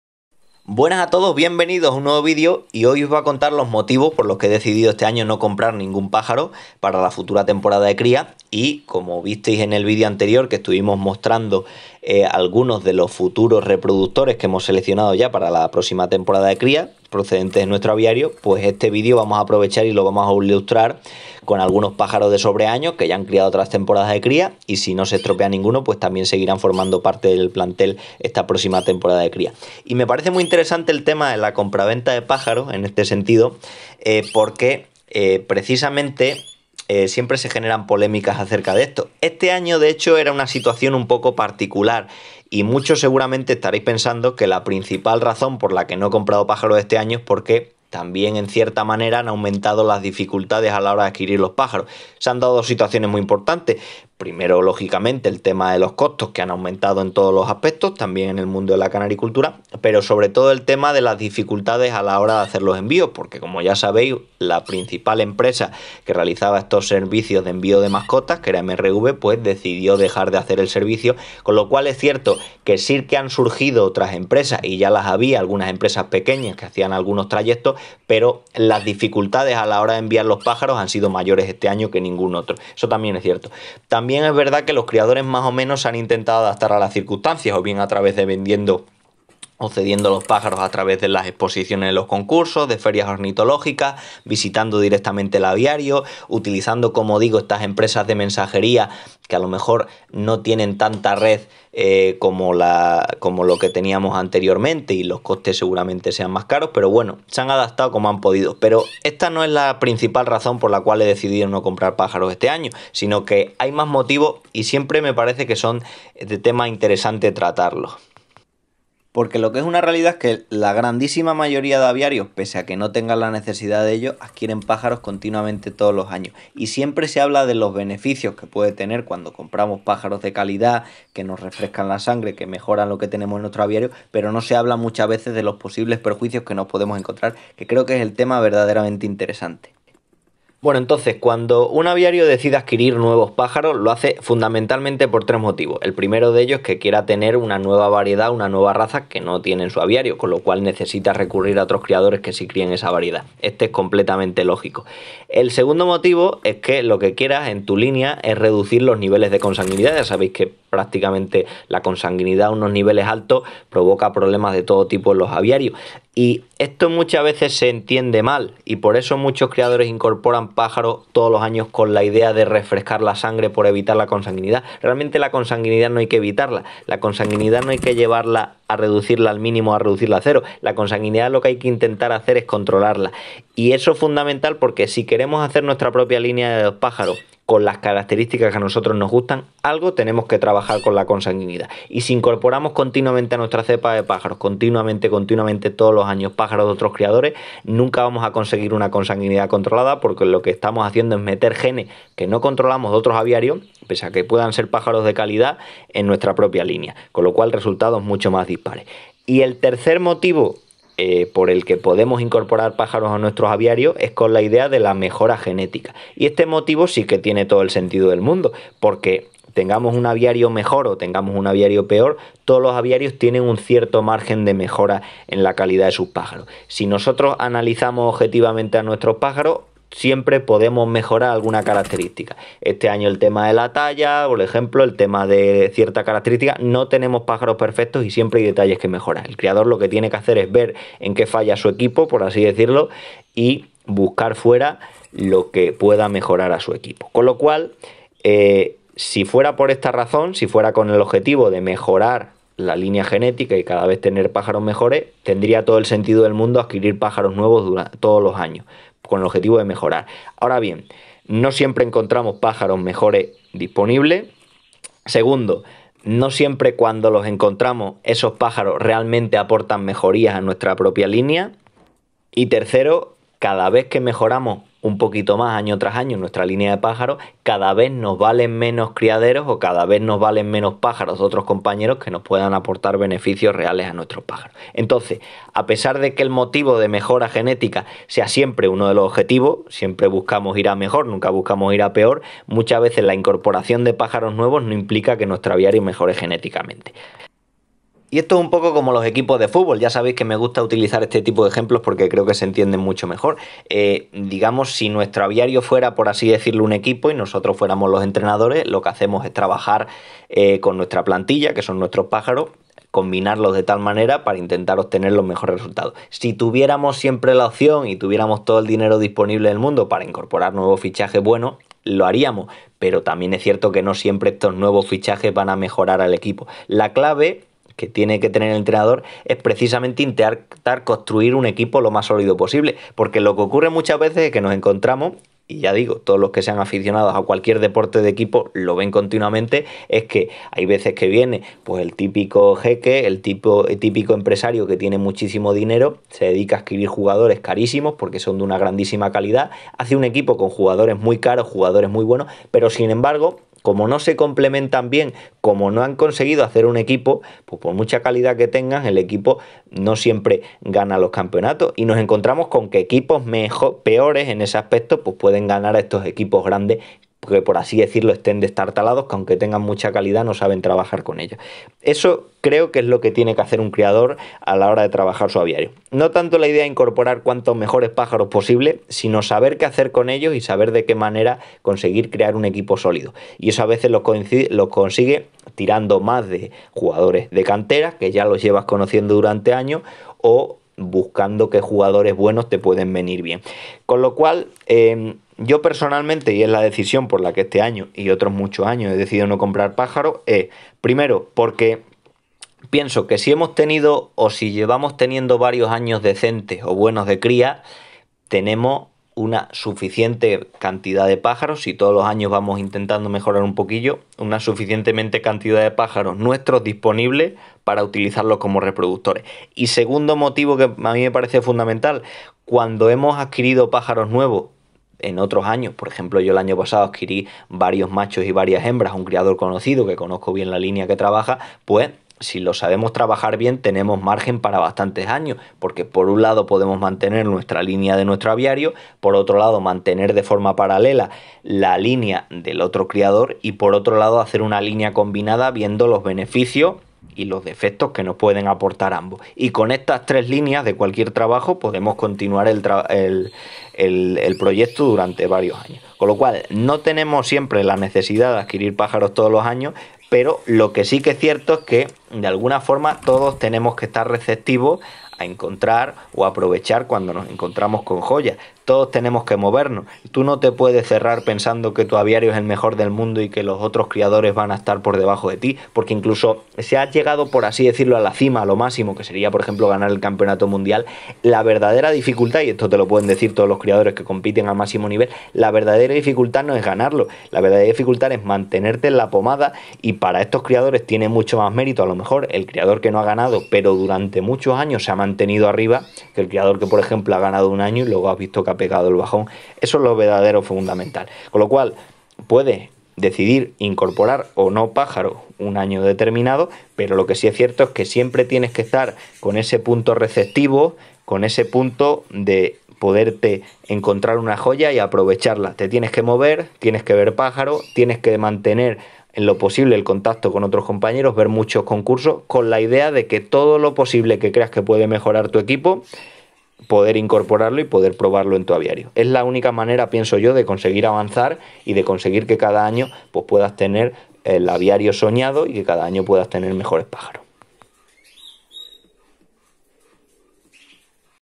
¿POR QUÉ NO COMPRO PÁJAROS ESTE AÑO?
Este año he decidido no comprar ningún pájaro y os quiero contar los motivos. ¿Opináis como yo?